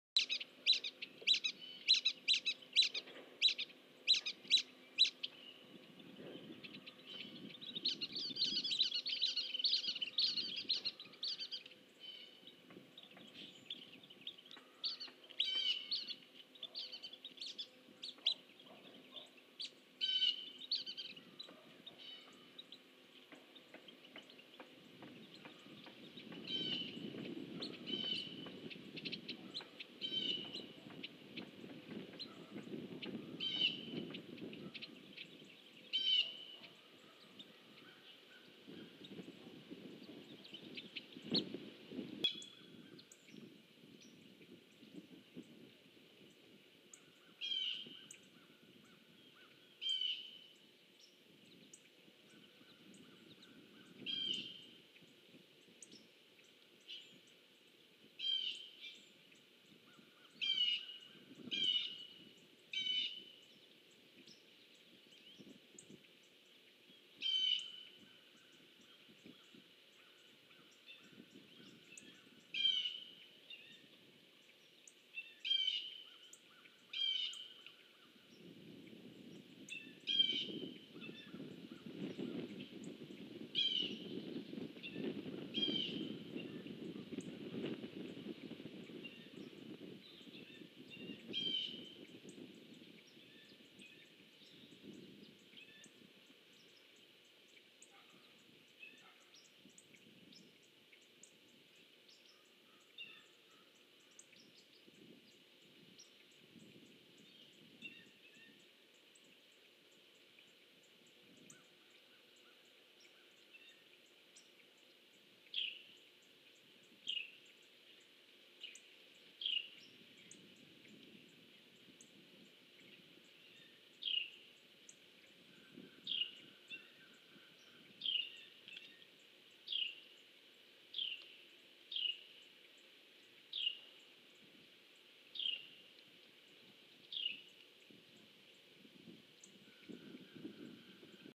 Video 2015 02 03 112257 Great Horned Owl 2:22 pm
Description

Location: Savannah, Georgia

Camera Host: Skidaway Audubon
preening and owl sounds